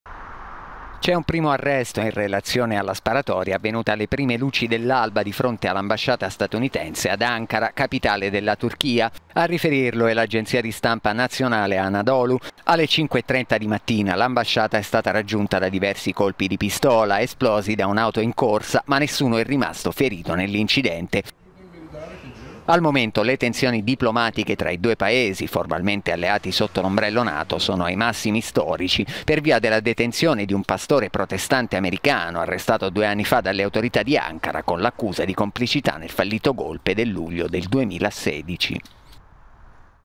Turchia, primo arresto per gli spari contro ambasciata Usa
Alle 5 di mattina, l'ambasciata è stata raggiunta da diversi colpi sparati da un auto in corsa …